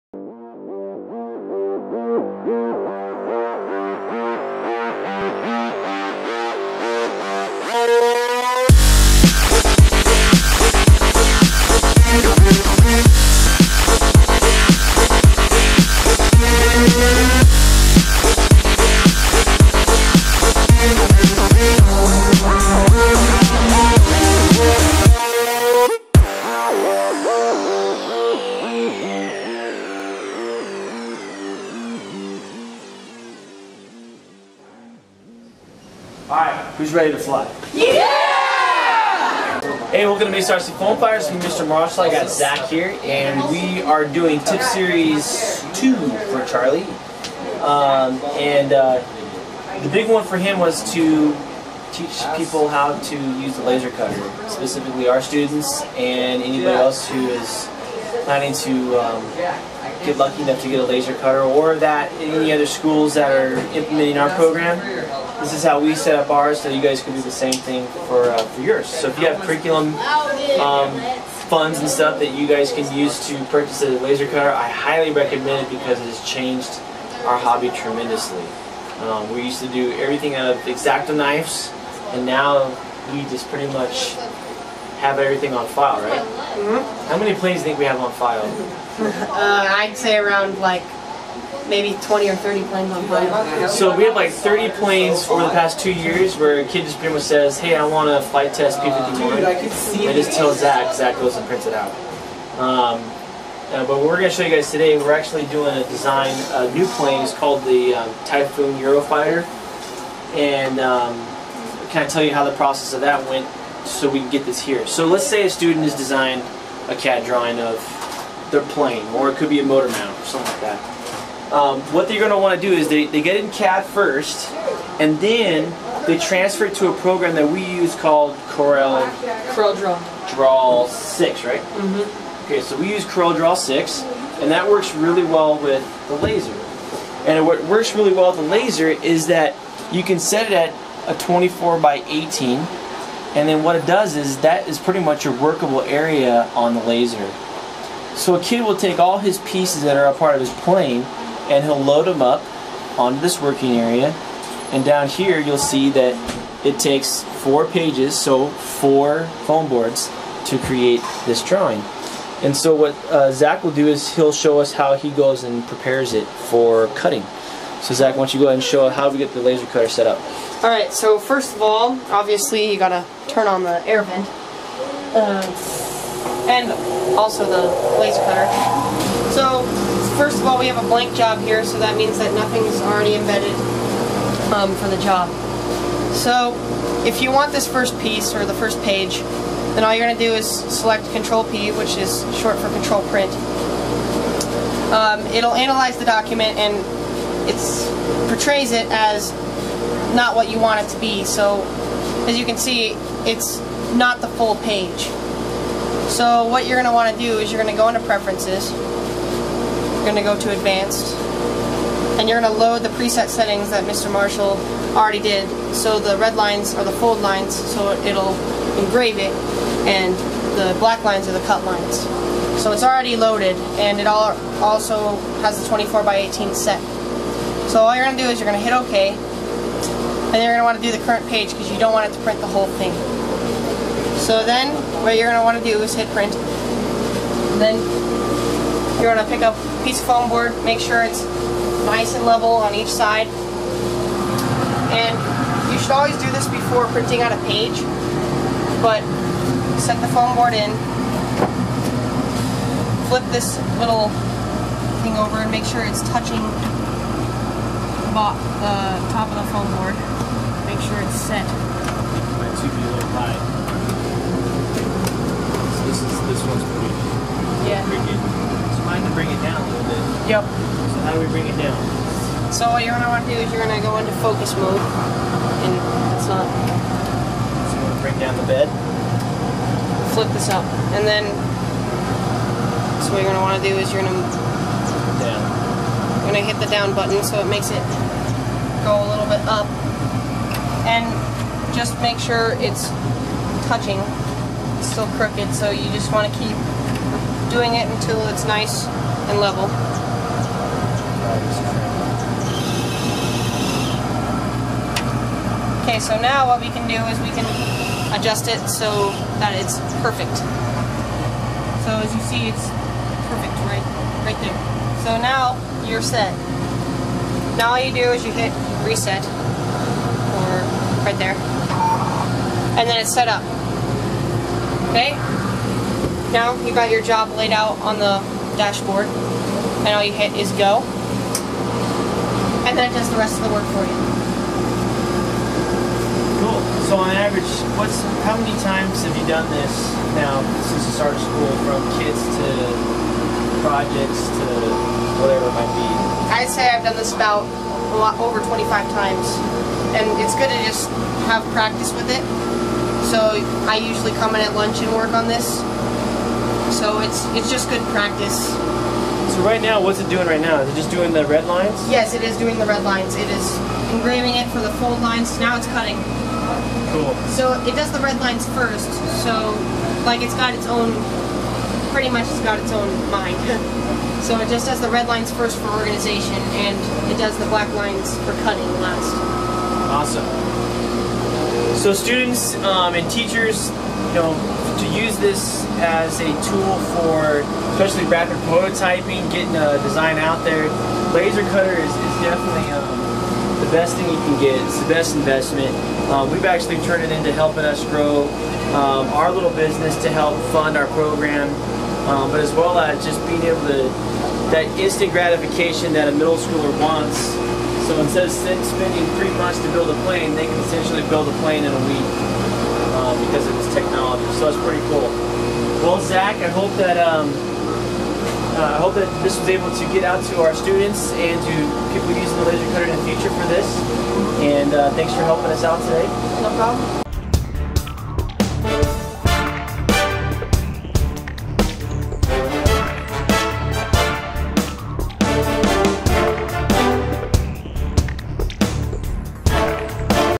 MESArcFF - Laser Cutter - TIP SERIES
Episode 2 of TIP SERIES,

EPS 9 S5
This episode features Product Manager Zach demonstrating the usage of the Epilog Laser Cutter and Engraver.

This is the second installment of the Tip Series directed by Charlie our 8th grade Chief Editor, if there is something that is basic and beneficial for students to know how to do as beginner builder and flyer please comment and leave suggestions below on what you would like help on.